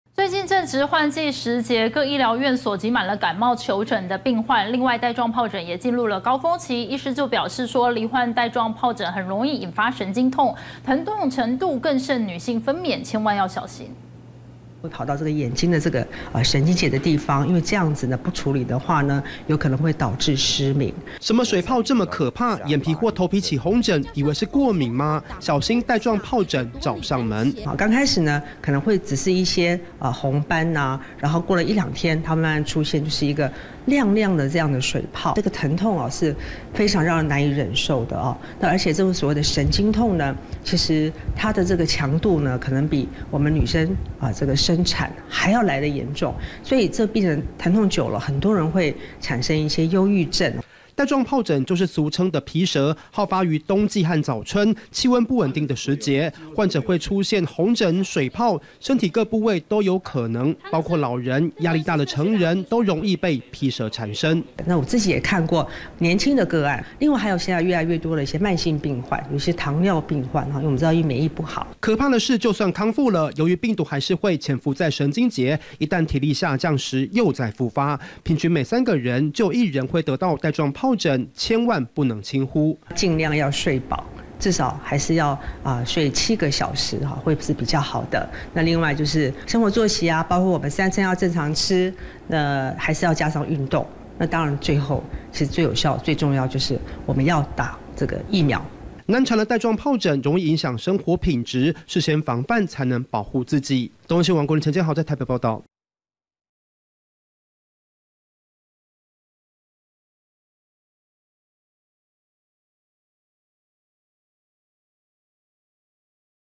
換季時節皮蛇出沒！身體長紅疹水泡勿輕忽
最近正值換季時節，各醫療院所擠滿感冒求診病患，另外帶狀疱疹也進入高峰期。醫生表示，罹患帶狀皰疹很容易引發神經痛，疼痛程度更甚女性分娩，千萬小心。